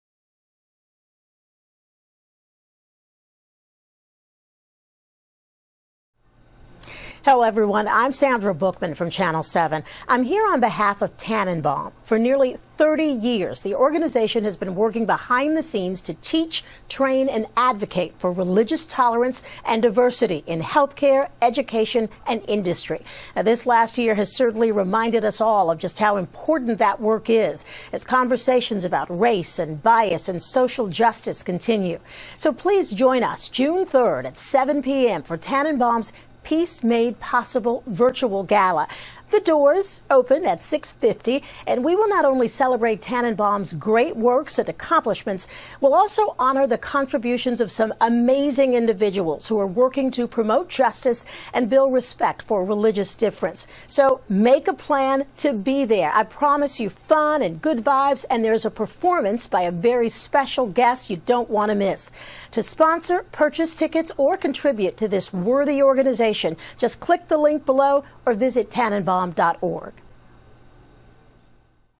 A Message from Sandra Bookman!
Sandra Bookman, anchor at WABC-TV, has a special announcement about our 2021 Peace Made Possible Virtual Gala!